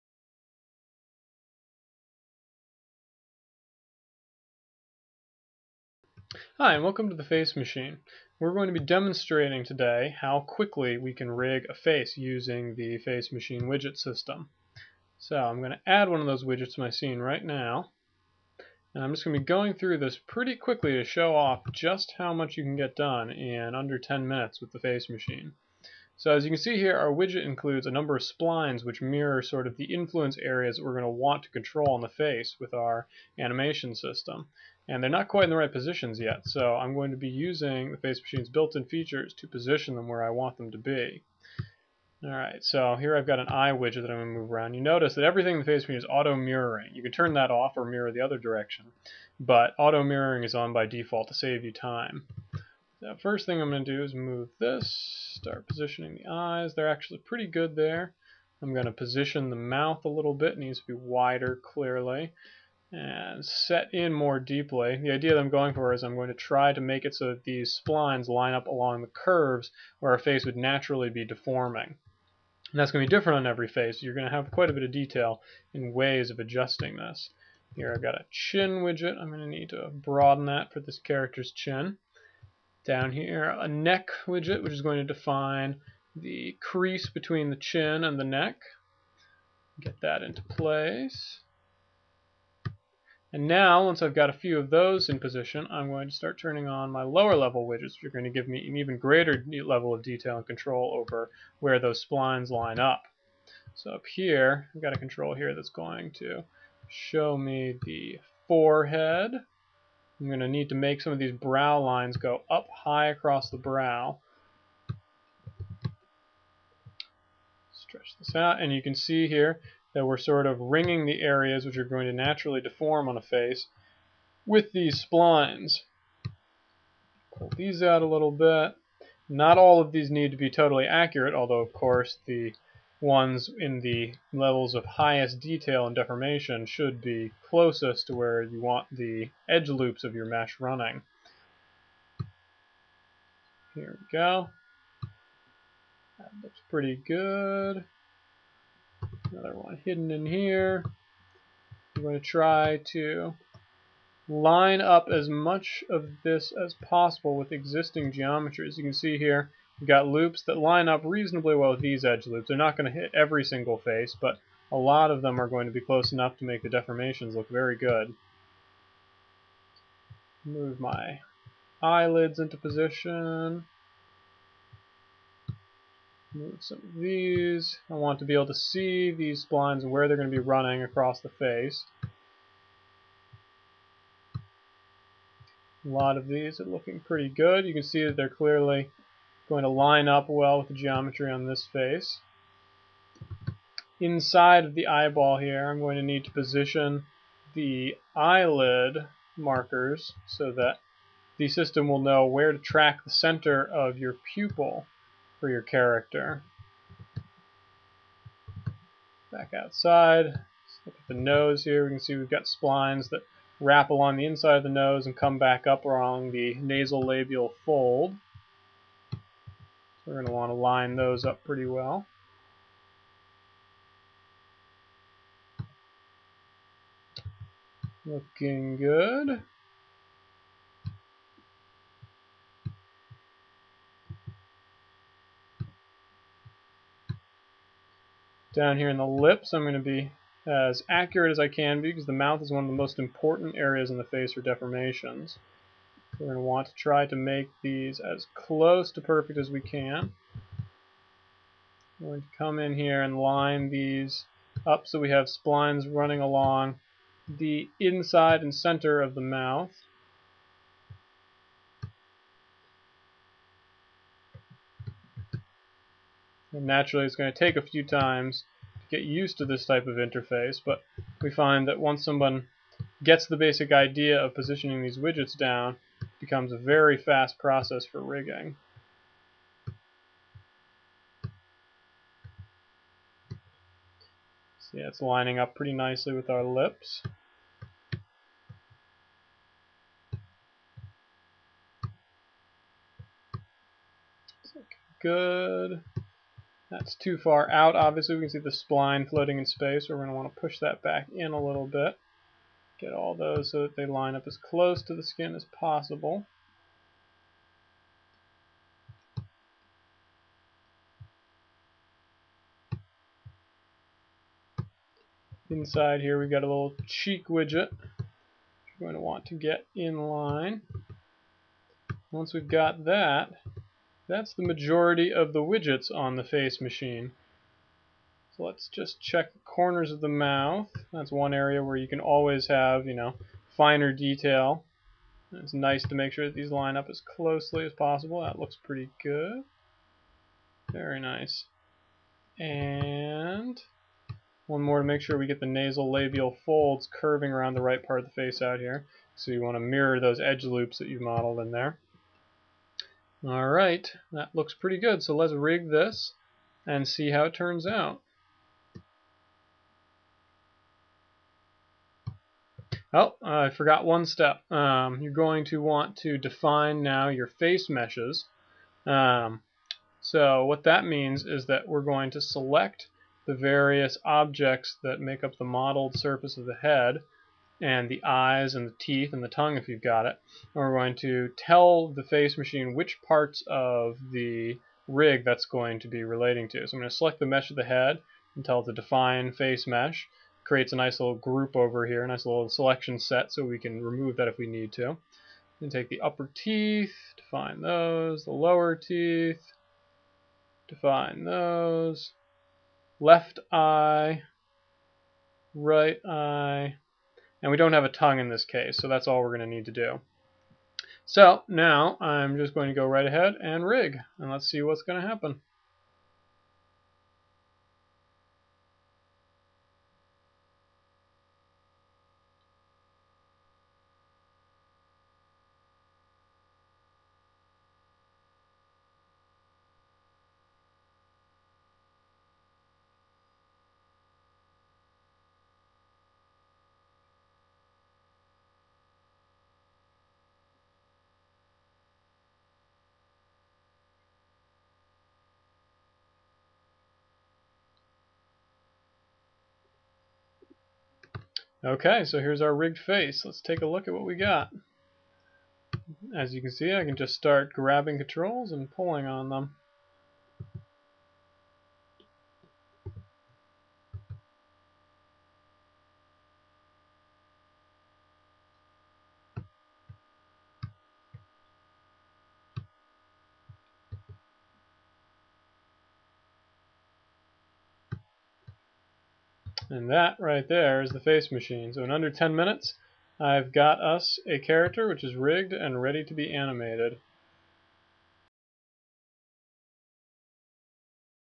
The Face Machine - Part 1 Quick Rigging
The Face Machine is a facial rigging plug-in for Maya.  The Face Machine uses a simple "face widget" to rig your character.  Just fit it to your mesh and hit rig!!